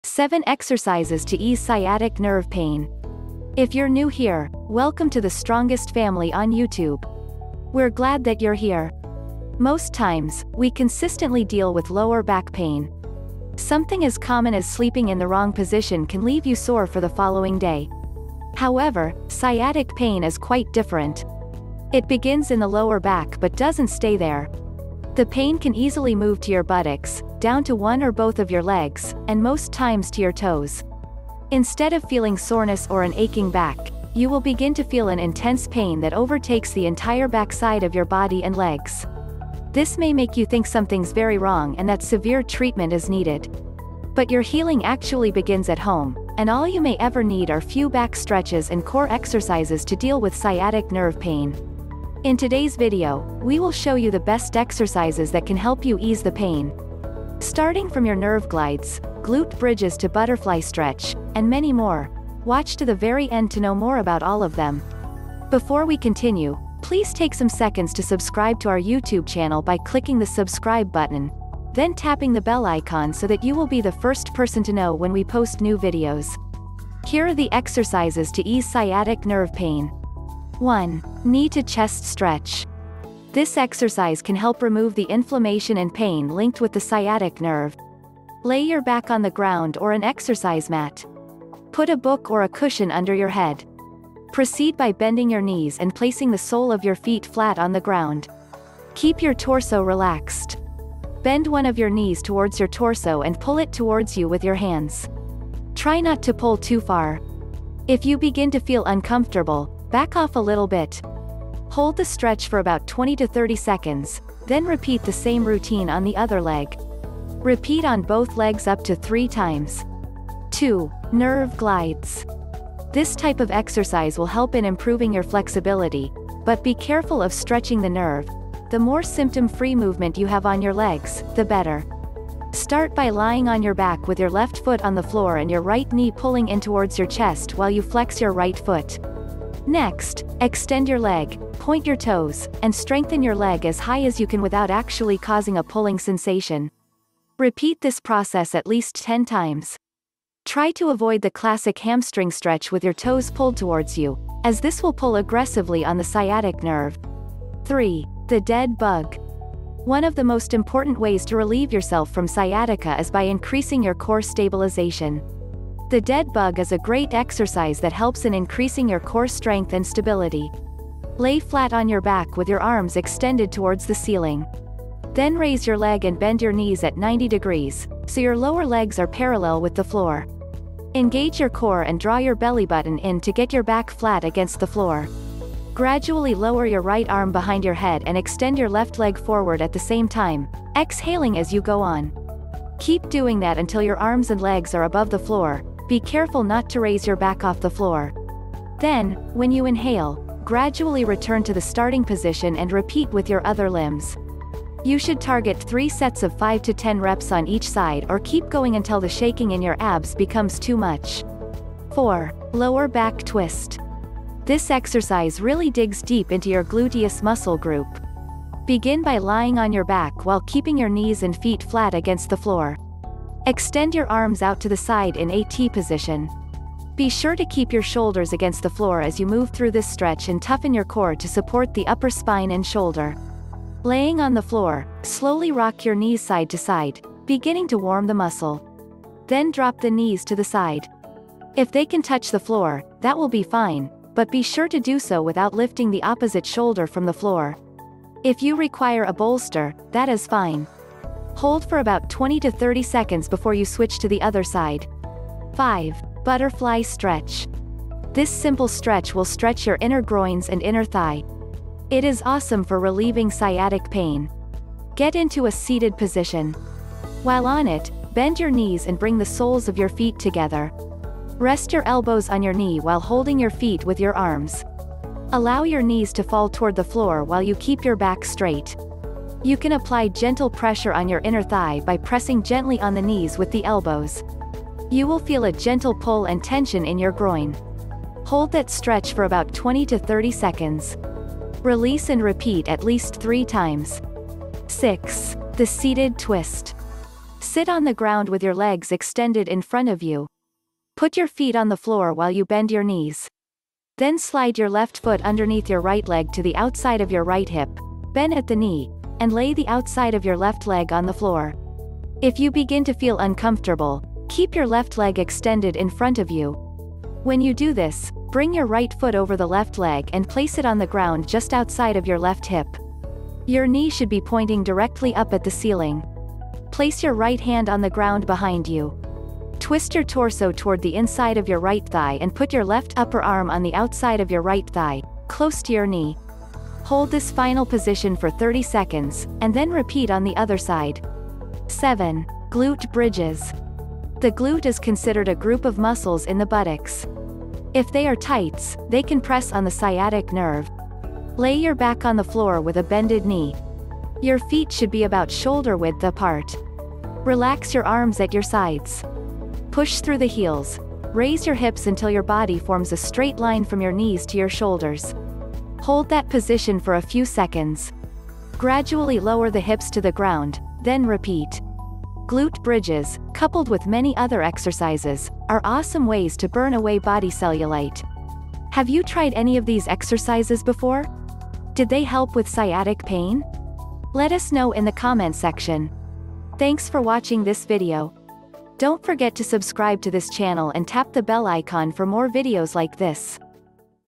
7 Exercises To Ease Sciatic Nerve Pain (Cure Sciatic Pain)
Sciatic Nerve Pain | 7 Exercises to Ease Sciatic Nerve Pain.

We consistently deal with lower back pain. Something as common as sleeping in the wrong position can leave you sore for the following day.

However, sciatic pain is quite different. It begins in the lower back but doesn’t stay there. The pain can easily move to your buttocks, down to one or both of your legs, and most times to your toes.

Instead of feeling soreness or an aching back, you will begin to feel an intense pain that overtakes the entire backside of your body and legs. This may make you think something’s very wrong and that severe treatment is needed.

But your healing actually begins at home, and all you may ever need are few back stretches and core exercises to deal with sciatic nerve pain.

Here’re The Exercises to Ease Sciatic Nerve Pain:

1. Knee to Chest Stretch:

This exercise can help remove the inflammation and pain linked with the sciatic nerve.

2. Nerve Glides:

This type of exercise will help in improving your flexibility, but be careful of stretching the nerve — the more symptom-free movement you have on your legs, the better.

3. The Dead Bug:

One of the most important ways to relieve yourself from sciatica is by increasing your core stabilization. The dead bug is a great exercise that helps in increasing your core strength and stability.

4. Lower Back Twist:

This exercise really digs deep into your gluteus muscle group. Begin by lying on your back while keeping your knees and feet flat against the floor.

5. Butterfly Stretch:

This simple stretch will stretch your inner groins and inner thigh. It is awesome for relieving sciatic pain.

6. The Seated Twist:
This is an exercise awesome for relieving sciatic pain.

7. Glute Bridges:
The glute is considered a group of muscles in the buttocks. If they are tights, they can press on the sciatic nerve.